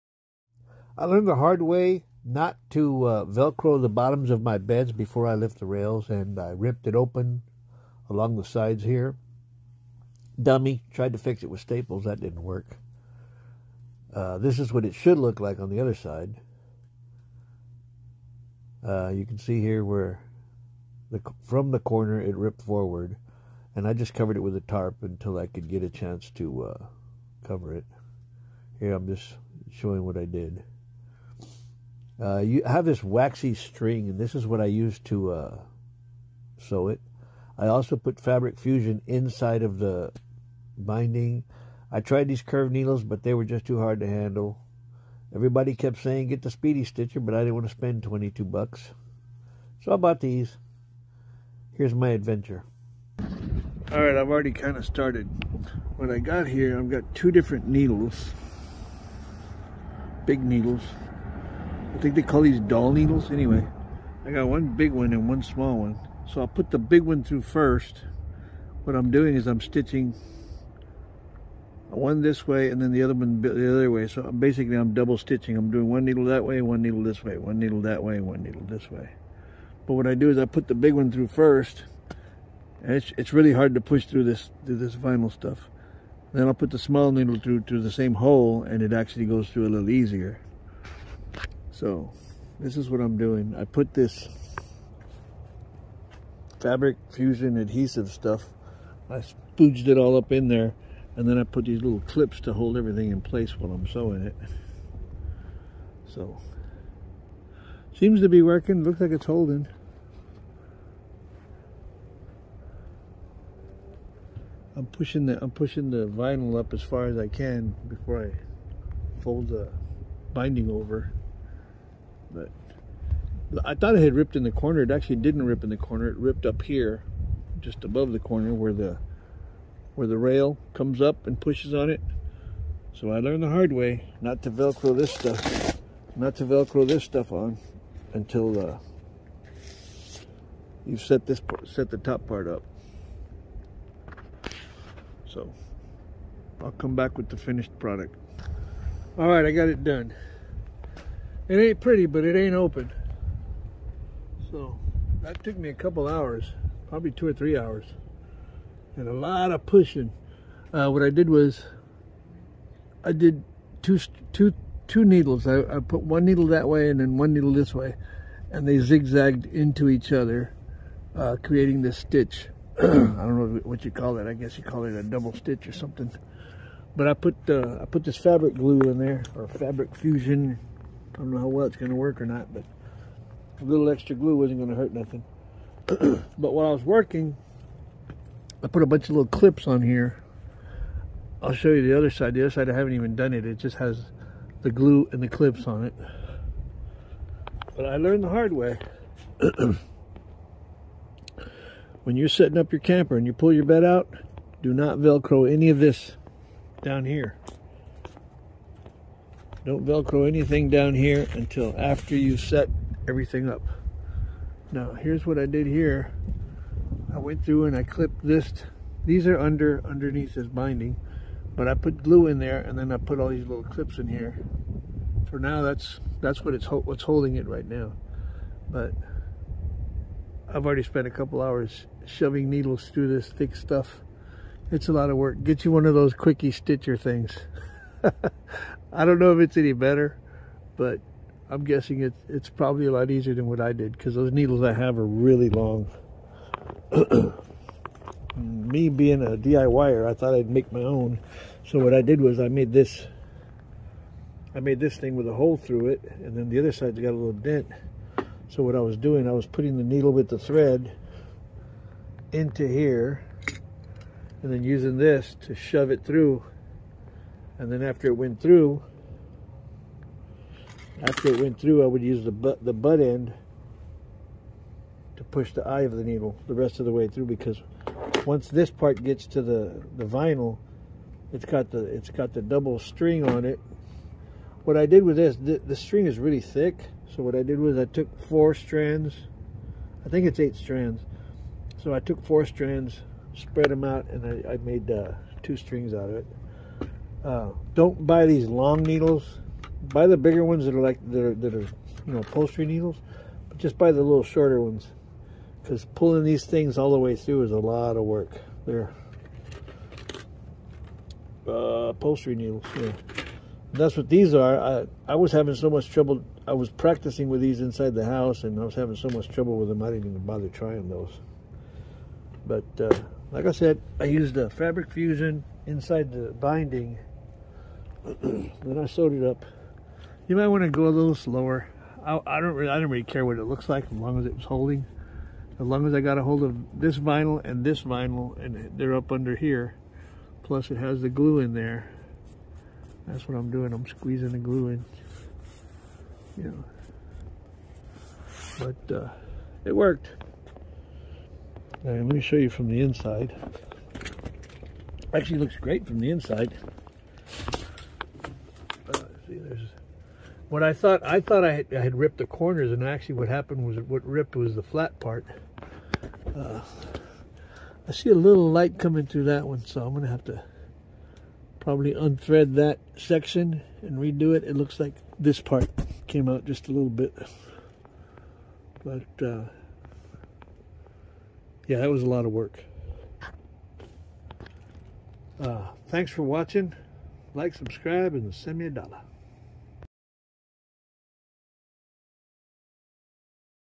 POP-UP CAMPER SEAM STITCHING (NOT) A DIY HOW-TO!
I learned the hard way not to Velcro the bottom of my beds after I've pulled them out.
 You should put the rails up 1st and then go outside and Velcro the bottoms in place.
GO BUY A SPEEDY STITCHER!
I was too hard headed and tried to DIY my own stitching tool... It worked but was probably harder than using a proven tool.
I'm also wondering if leaving it set-up might work better than laying it down the way I did in this video.
I'll do an "Update / Part 2" video after I trying to stitch it while set-up on the other side.